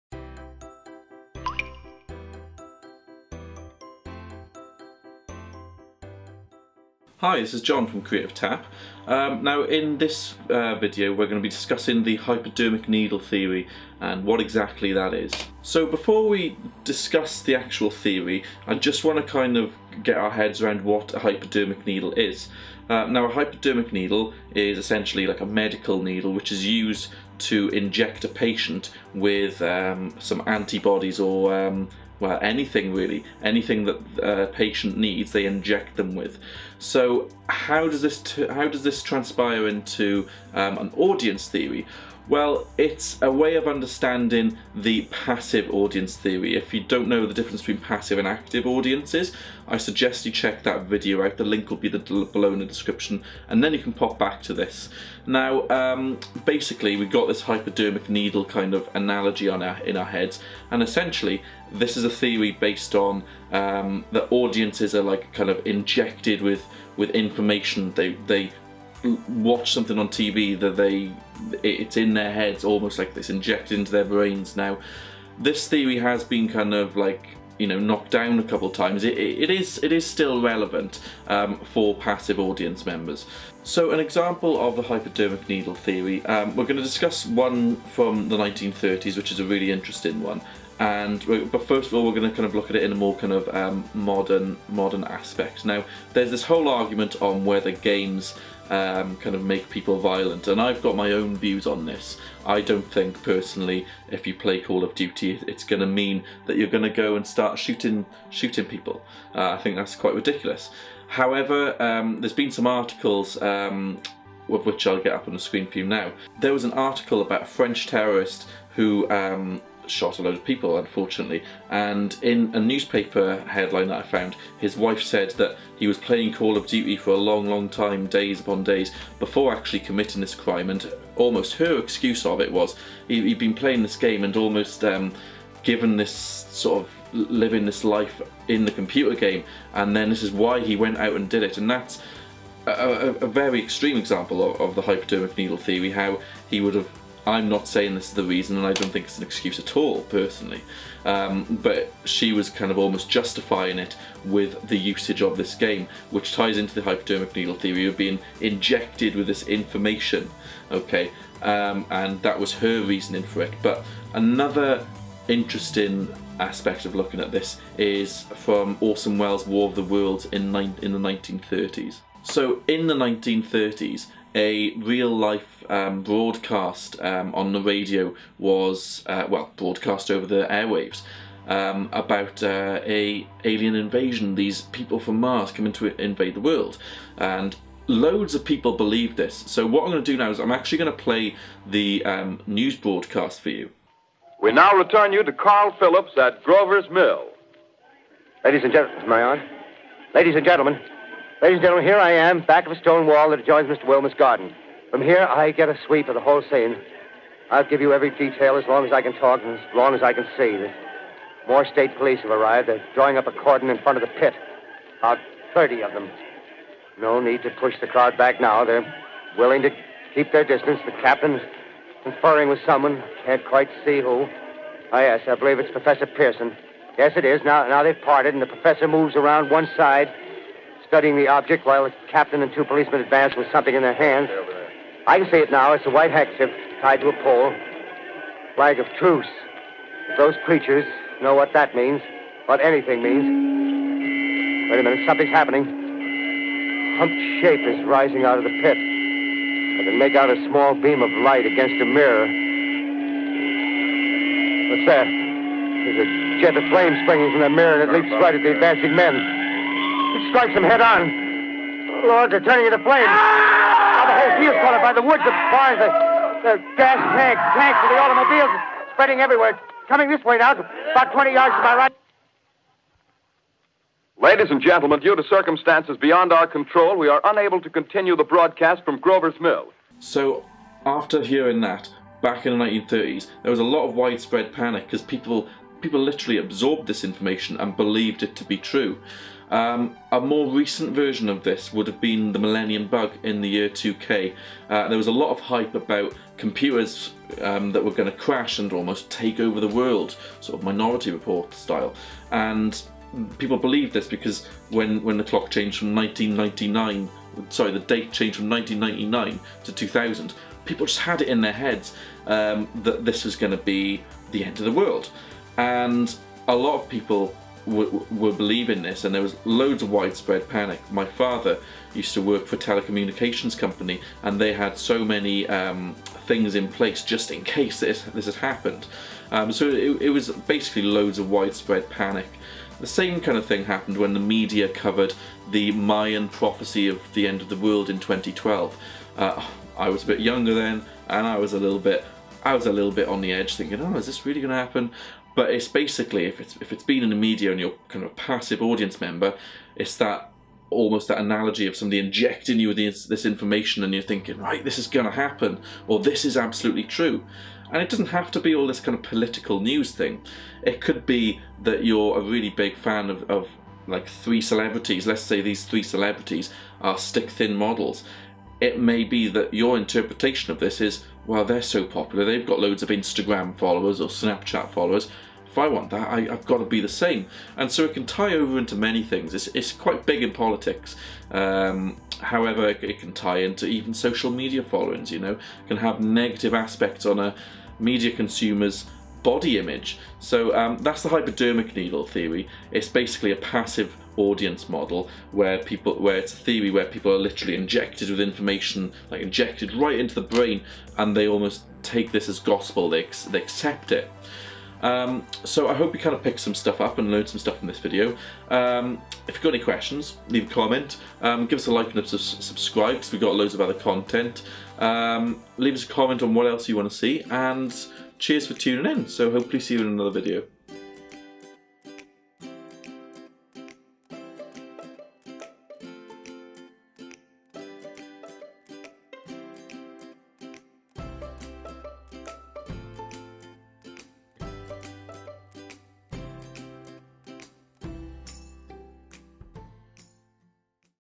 Hypodermic Needle Theory Explained
This video will explain the Hypodermic needle theory with examples!

Join us on instagram at @creative.tap